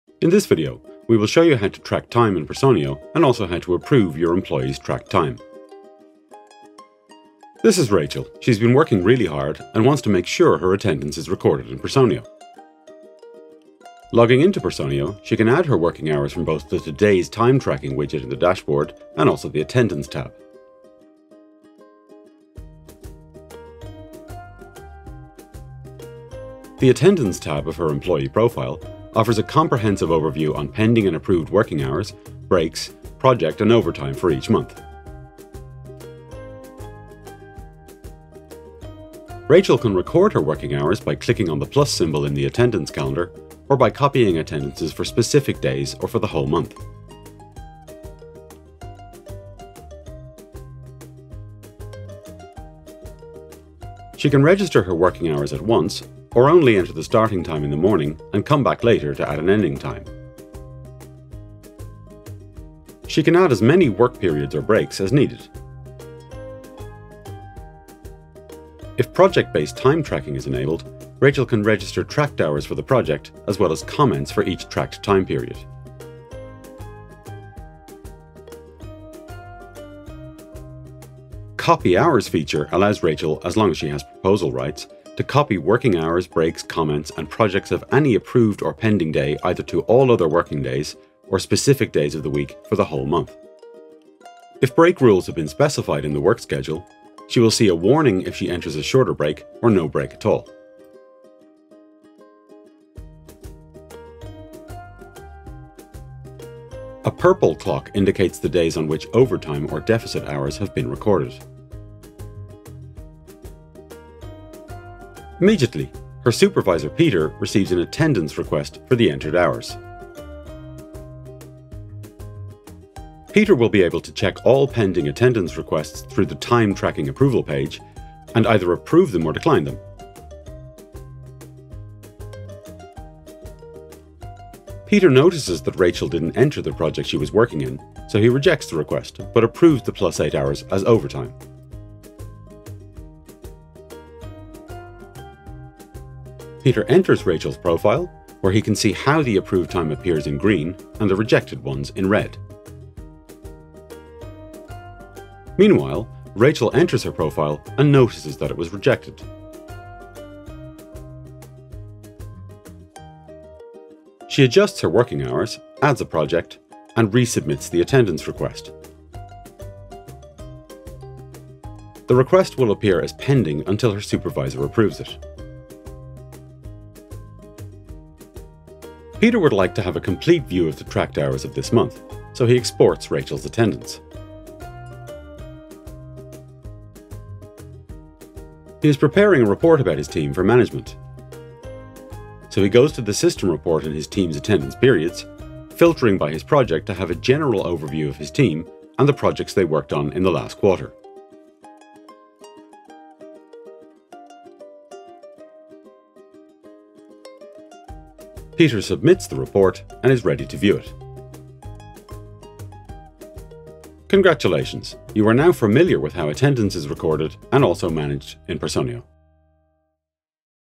Tracking Attendance in Personio
In this video, we will show you how to track time in Personio, and also how to approve your employee's tracked time.

00:00 Recording Attendance in Personio
01:56 Approving Attendances
02:59 Reporting Attendance

At Personio, we make HR professionals real heroes by giving them more time to focus on what matters most: their employees.